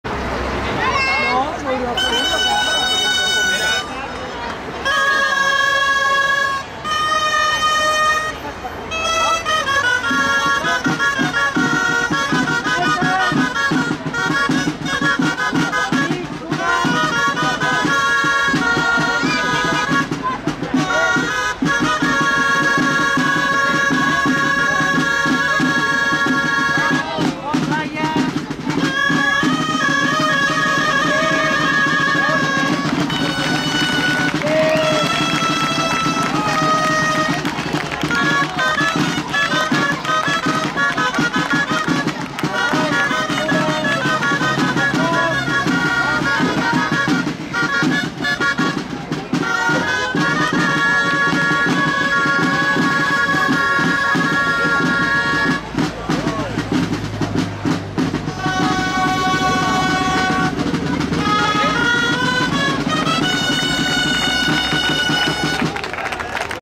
2014-10-25. Capgrossos de Mataró, 2 Pilars de 4 (1), Mataró
Festes de Sant Simó, Camí Ral